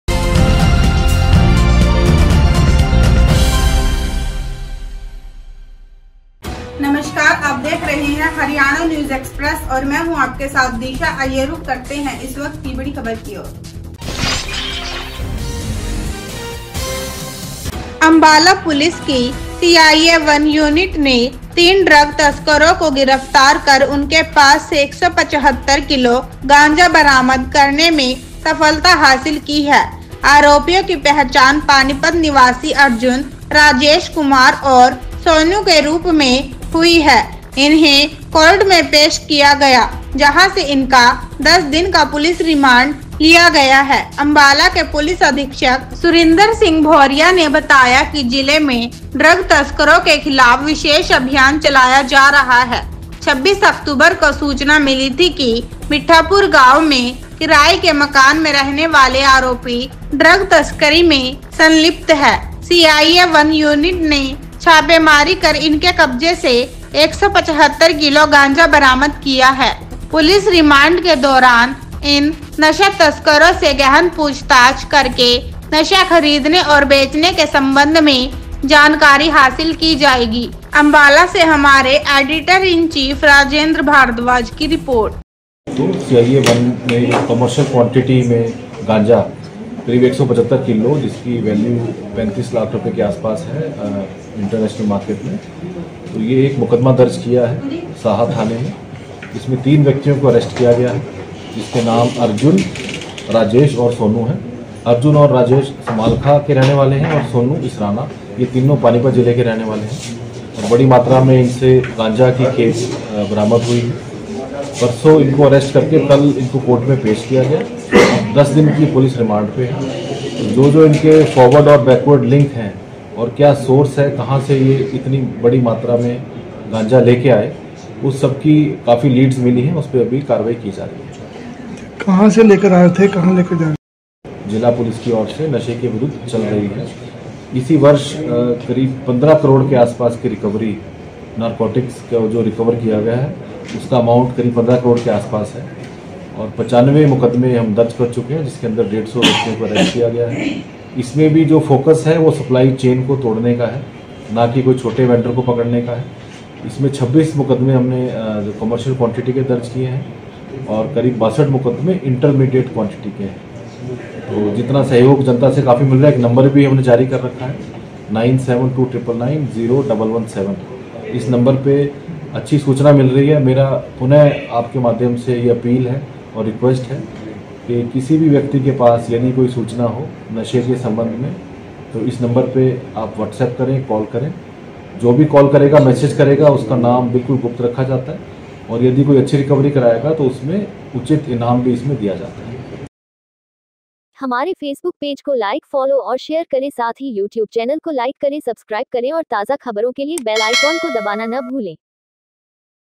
अम्बाला : 175 किलो गांजे समेत तीन नशा तस्कर गिरफ्तार
अम्बाला पुलिस की सीआईए 1 यूनिट ने तीन ड्रग तस्करों को गिरफ्तार कर उनके पास से 175 किलो गांजा बरामद करने में सफलता हासिल की है। आरोपियों की पहचान पानीपत निवासी अर्जुन, राजेश कुमार और सोनू के रूप में हुई है। इन्हें कोर्ट में पेश किया गया, जहां से इनका 10 दिन का पुलिस रिमांड लिया गया है। अम्बाला के पुलिस अधीक्षक सुरिंदर सिंह भोरिया ने बताया कि जिले में ड्रग तस्करों के खिलाफ विशेष अभियान चलाया जा रहा है। 26 अक्टूबर को सूचना मिली थी कि मिट्ठापुर गांव में किराए के मकान में रहने वाले आरोपी ड्रग तस्करी में संलिप्त हैं। सीआईए 1 यूनिट ने छापेमारी कर इनके कब्जे से 175 किलो गांजा बरामद किया है। पुलिस रिमांड के दौरान इन नशा तस्करों से गहन पूछताछ करके नशा खरीदने और बेचने के संबंध में जानकारी हासिल की जाएगी।

यह वाणिज्यिक मात्रा थी और साहा थाने में एनडीपीएस एक्ट के तहत मामला दर्ज किया गया था। रिमांड के दौरान सोर्स सप्लायर और ग्राहकों के बारे में और जानकारी हासिल की जाएगी और उनके खिलाफ भी कार्रवाई की जाएगी। हम सप्लाई चेन तोड़ने पर ध्यान दे रहे हैं।" "आरोपी रिश्तेदार हैं। अर्जुन को पहले एनडीपीएस मामले में गिरफ्तार किया गया था और उसके खिलाफ पानीपत में भी मामले दर्ज हैं। बरामद गांजे की बाजार कीमत करीब 35 लाख रुपये है। जनवरी 2024 से अब तक अंबाला पुलिस ने 95 मामलों में 150 ड्रग तस्करों को गिरफ्तार किया है और 15 करोड़ रुपये के नशीले पदार्थ बरामद किए हैं। अंबाला एसपी ने कहा: "नशीली दवाओं का सेवन एक सामाजिक समस्या है और तस्कर आम लोगों को निशाना बनाते हैं। हम अंबाला के लोगों से अपील करते हैं कि वे ड्रग सप्लायरों और अवैध गतिविधियों में शामिल अन्य लोगों के बारे में जानकारी पुलिस हेल्पलाइन नंबर पर साझा करें, ताकि तत्काल कार्रवाई की जा सके। उनकी पहचान गुप्त रखी जाएगी और उचित इनाम दिया जाएगा।